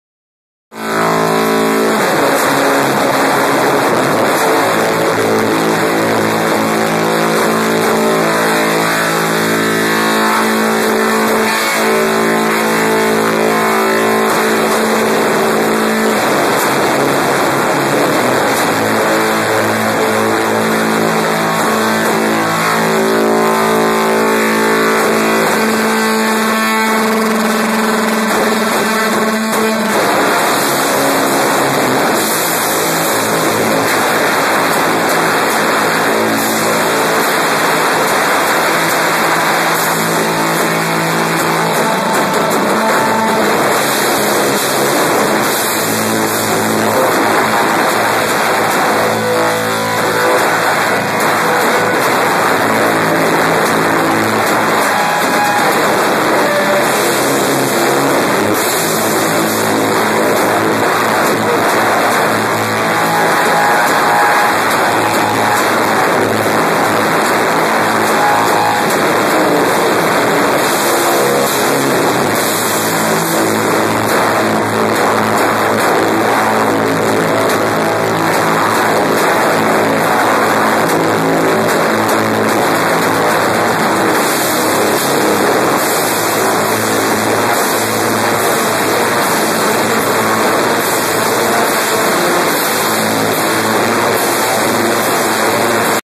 Thou @ undisclosed location, Wallingford, CT 10/06/11
Fuzzed out basement sludge. Watch the tom vibrating from the bass at the beginning.